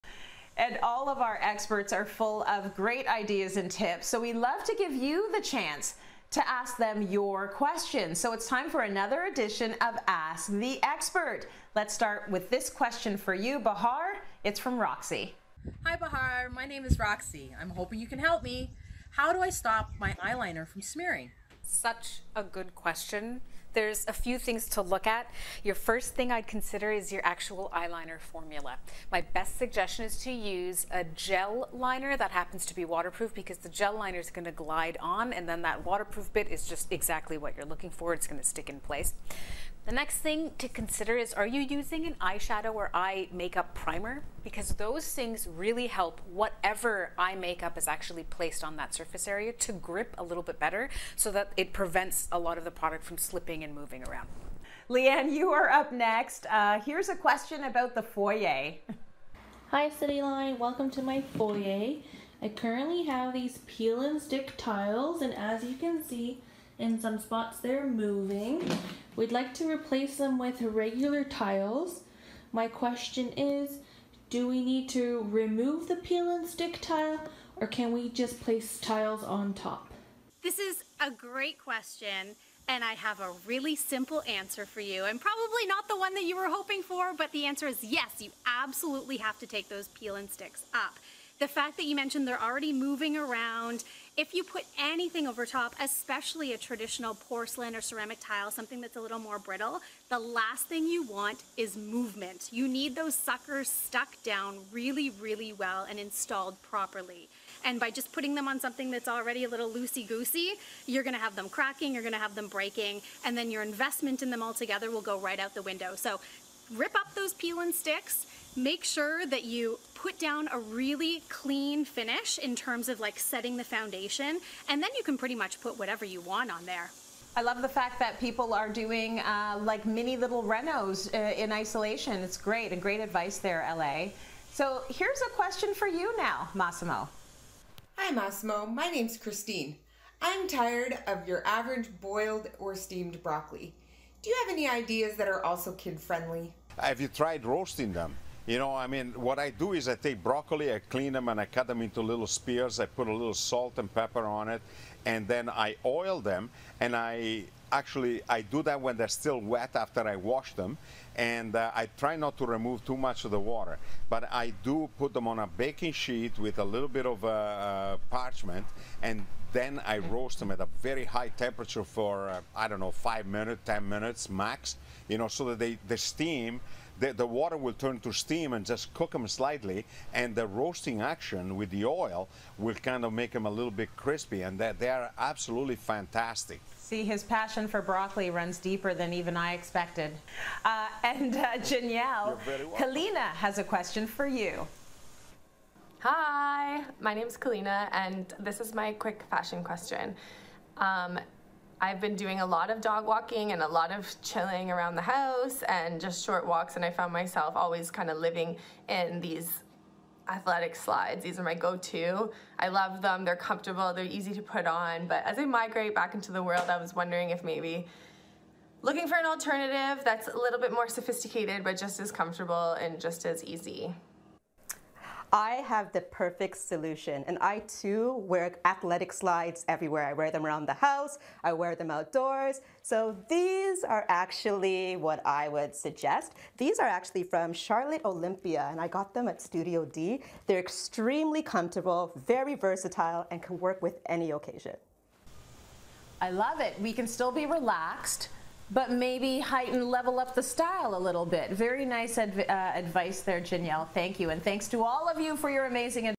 How to stop eyeliner smearing to tiling your foyer - we answer your questions
Our guest experts - Leigh-Ann, Massimo, Bahar and Janielle, answer YOUR questions!